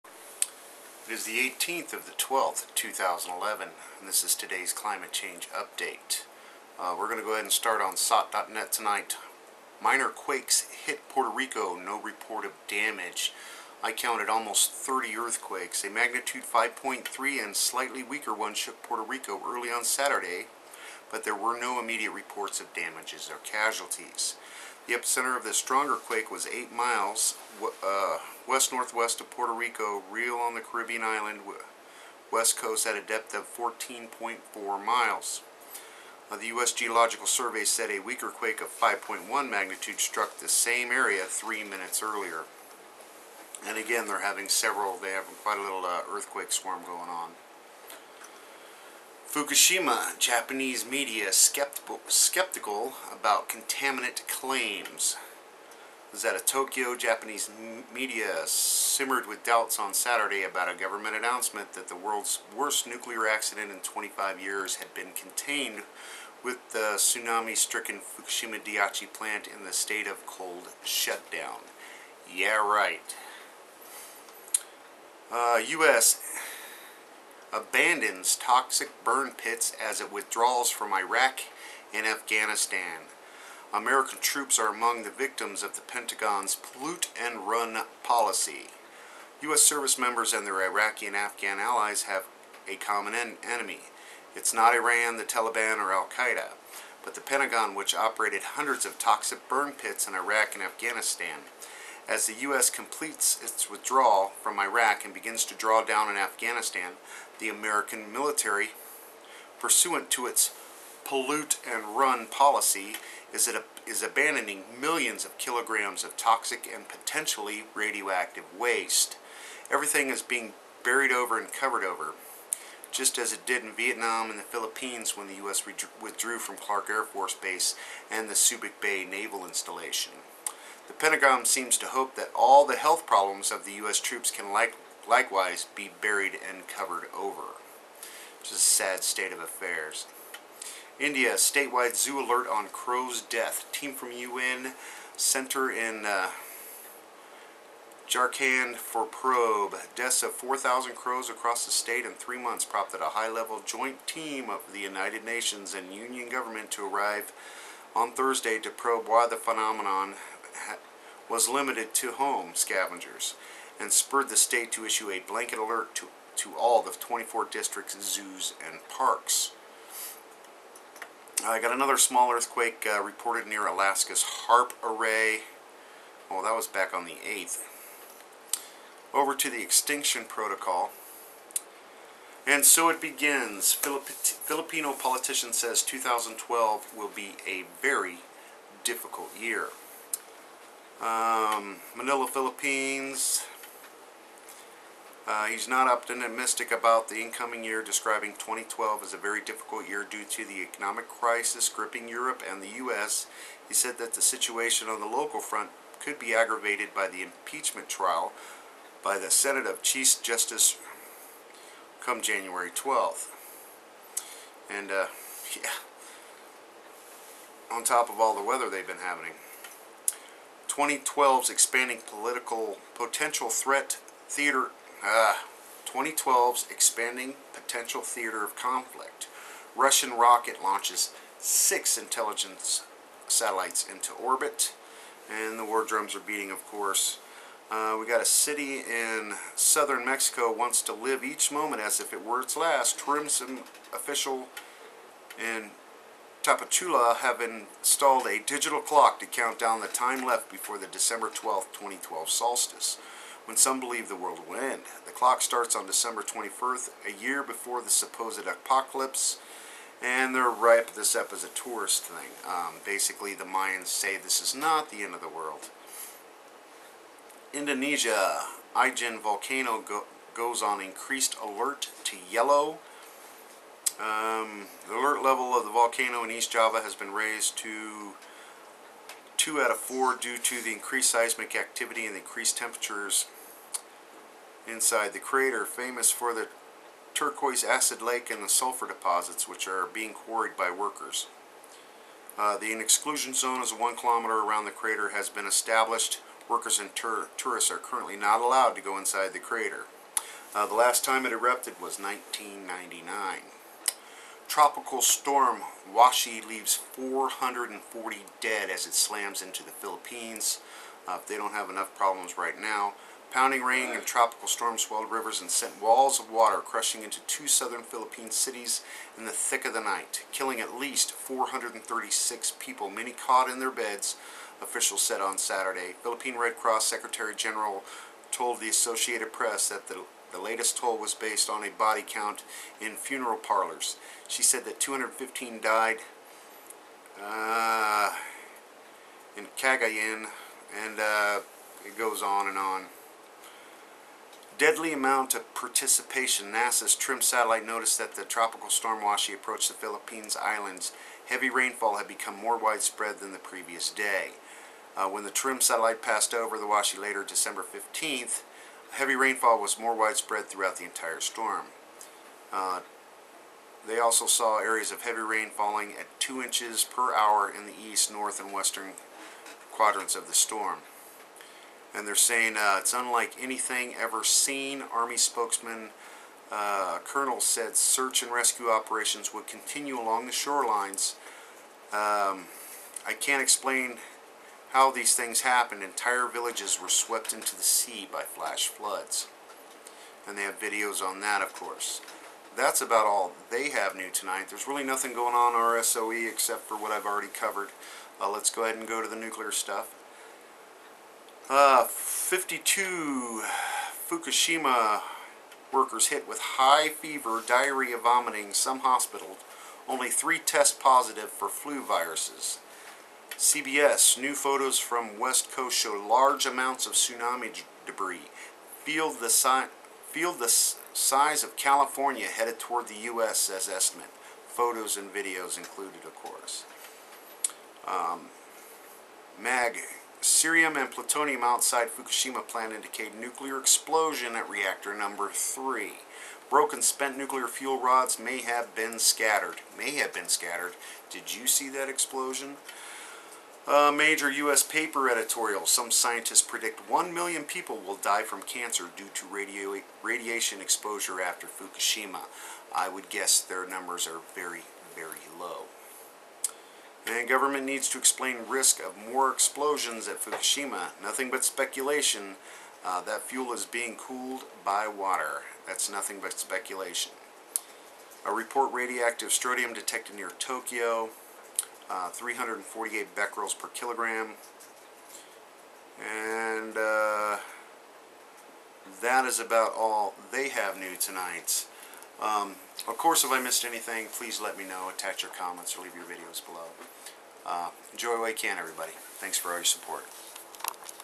Climate Change Update (18 December 2011) Puerto Rico Quake Swarm
A magnitude 5.3 earthquake and a slightly weaker one shook Puerto Rico early on Saturday, but there were no immediate reports of damage or casualties.
The epicentre of the stronger quake was 8 miles (13 km) westnorthwest of Puerto Real on the Caribbean island's west coast at a depth of 14.4 miles (23.2 km), the U.S. Geological Survey said. A weaker quake of 5.1 magnitude struck the same area three minutes earlier.

Tropical Storm Washi leaves 440 dead as it slams into the Philippines

CBS: New photos from west coast show large amounts of tsunami debris — Field the size of Calif. headed toward US, says estimate